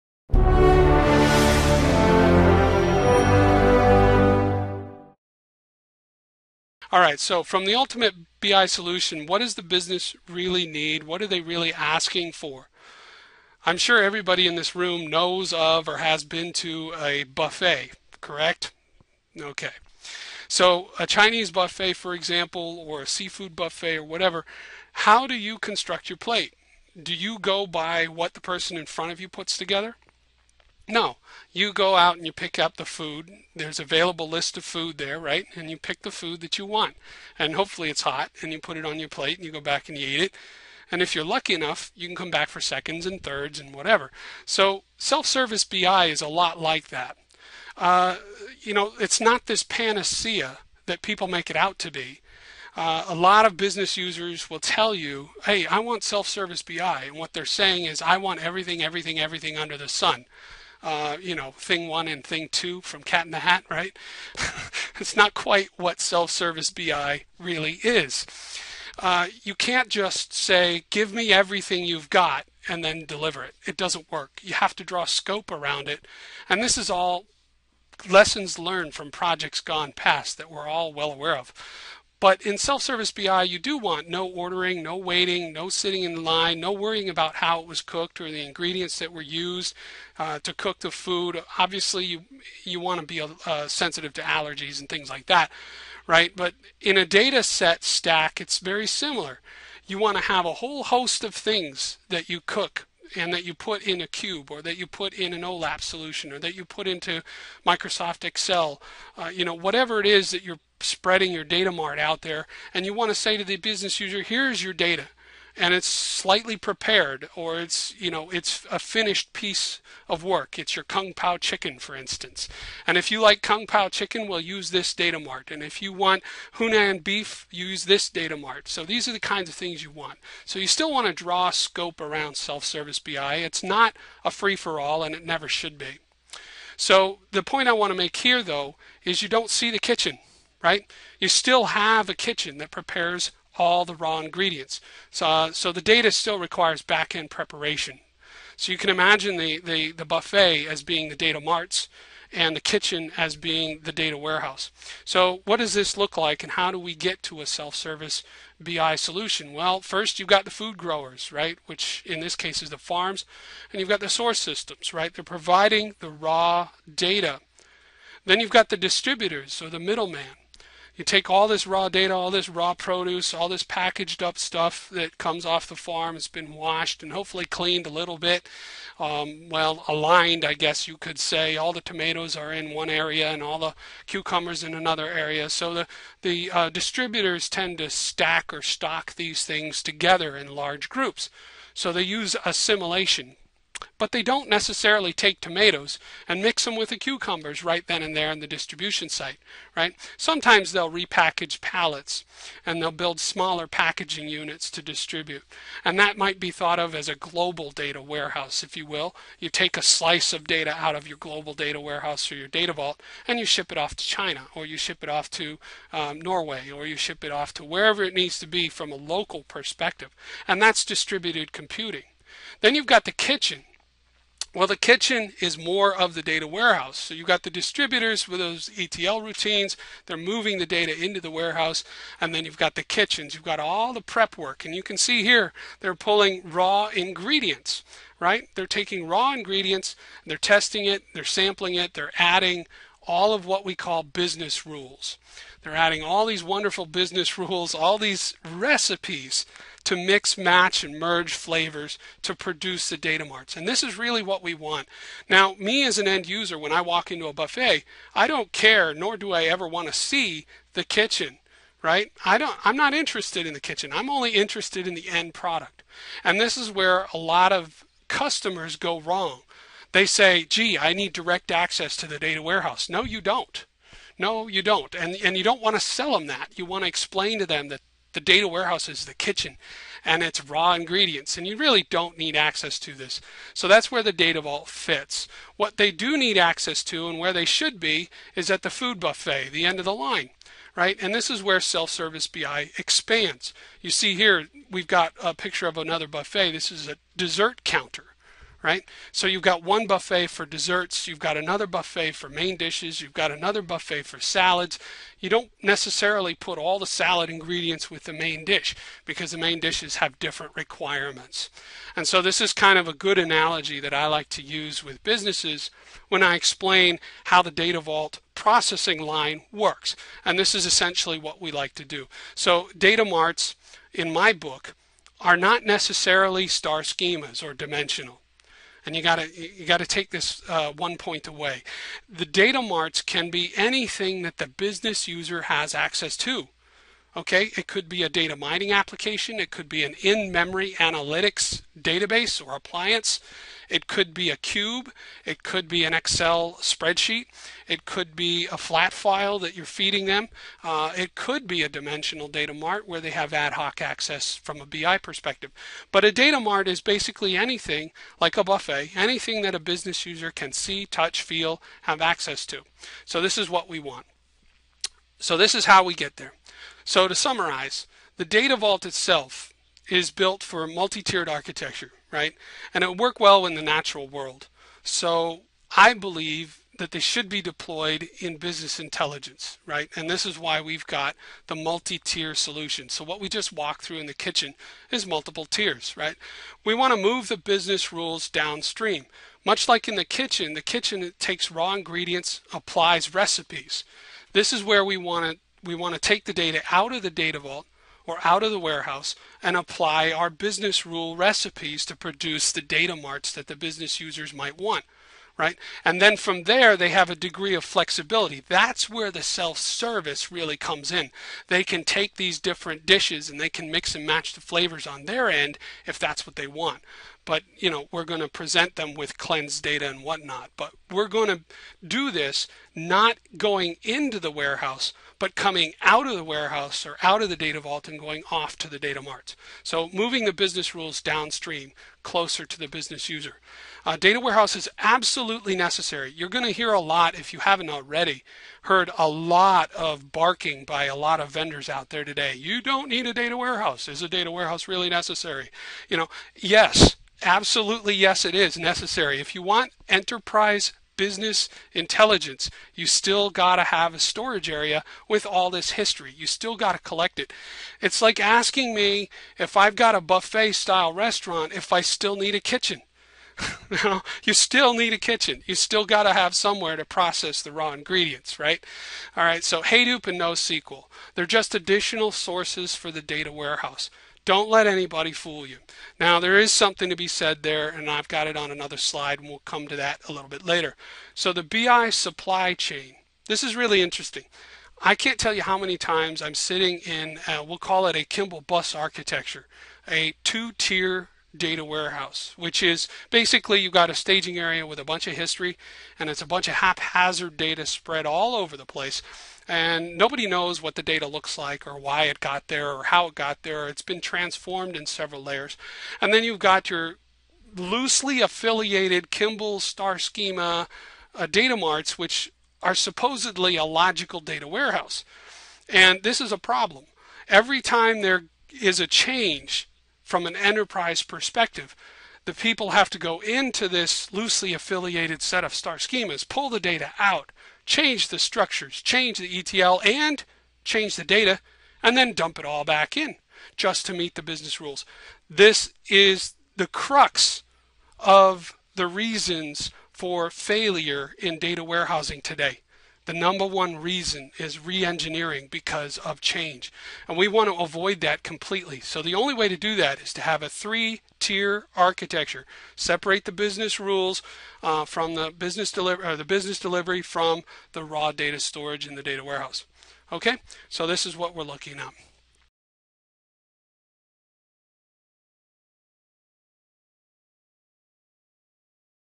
Data Vault Methodology - DW Architecture
Data Vault Methodology - The Ultimate Business Intelligence solution using a 3-tier Data Warehouse Architecture and the Data Vault using a simple analogy to explain to the business user.